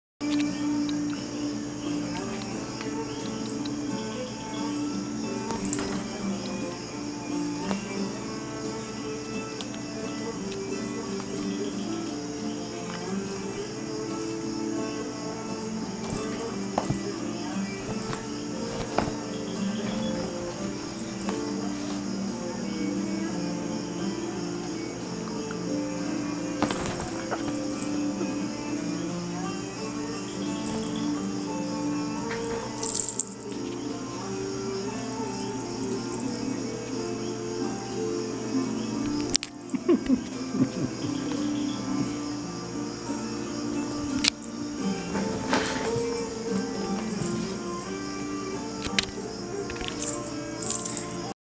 Dookie at night during hurricane Irma!
Dookie was not a bit nervous and was as playful as always under the sheets during the night hurricane Irma passed close to our island! He even stuck a nail in cat-dad's finger while playing, lol.